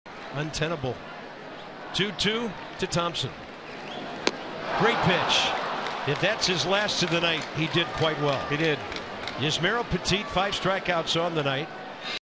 WSH@LAD: Petit fans Thompson to escape a jam in 6th
Yusmeiro Petit strikes out Trayce Thompson swinging to escape a jam in the bottom of the 6th inning